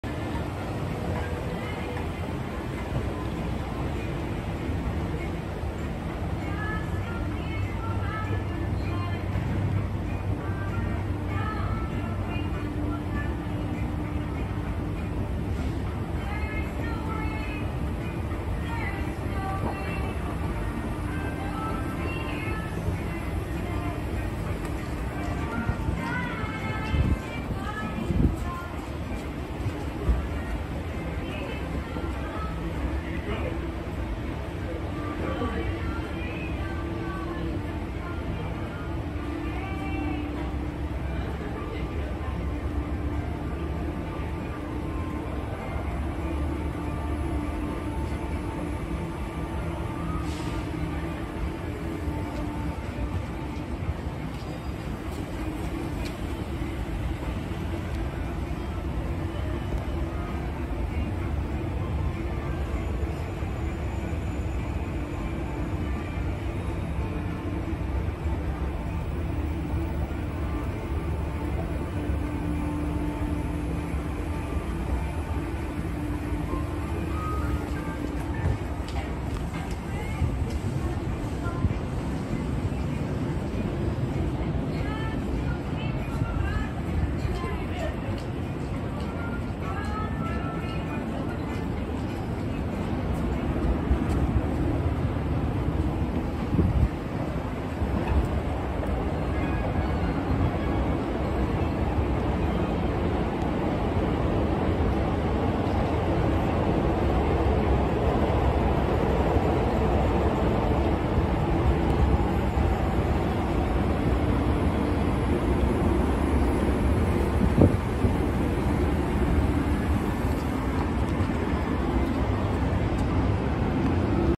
【Fujitec】NEWoMan横浜の屋外エスカレーターその2【2020年開業】escalator
2階から7階までのエスカレーターです。

楽しい面白いエスカレーターの動画をアップロードしてゆきます。
I am introducing my favorite Japanese escalators on this channel.